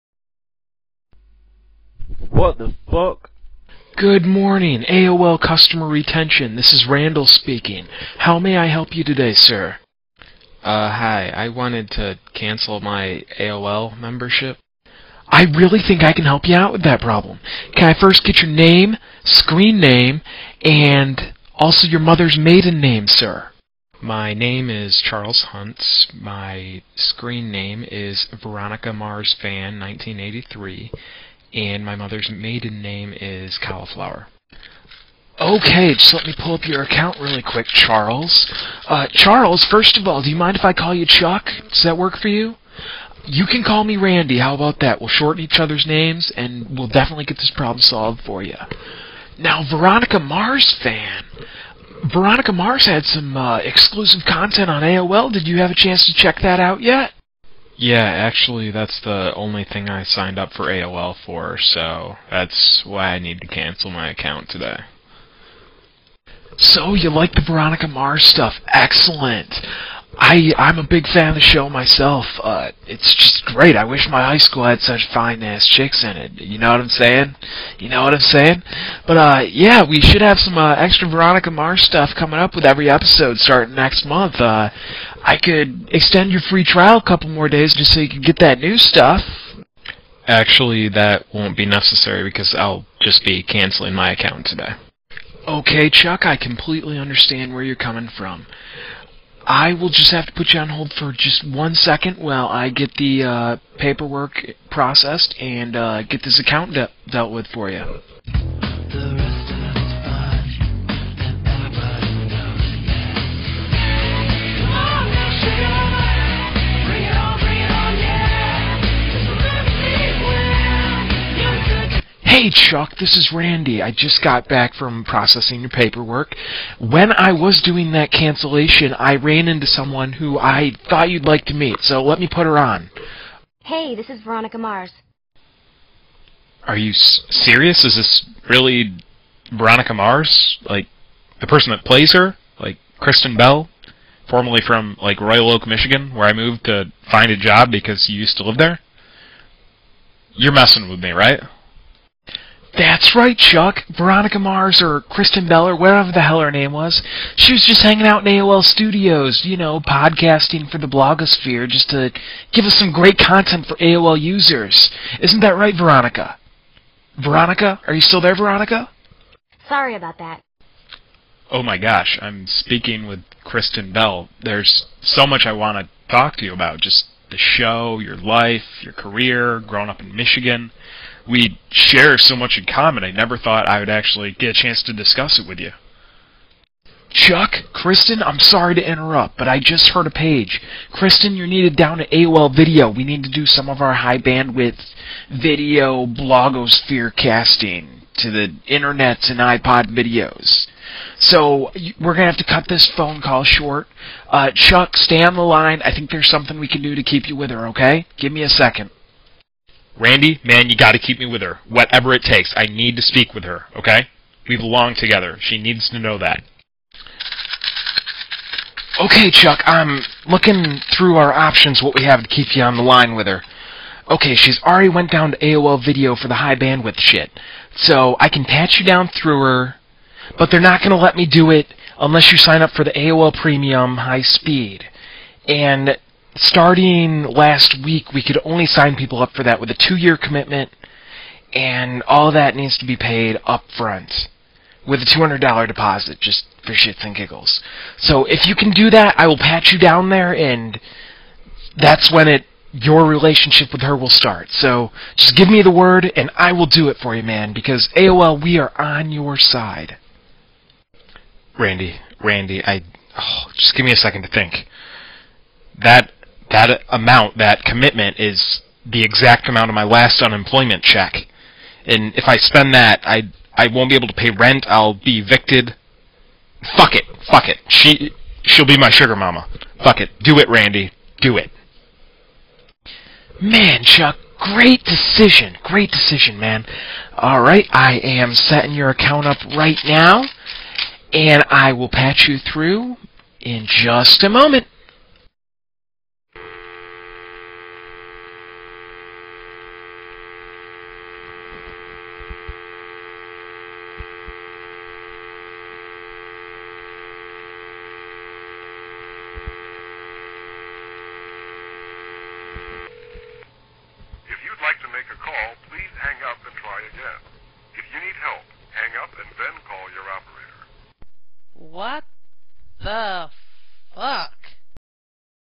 WTF Chuck - Cancel
Chuck decides to cancel an unnessary service in order to reduce his monthy costs. (4190)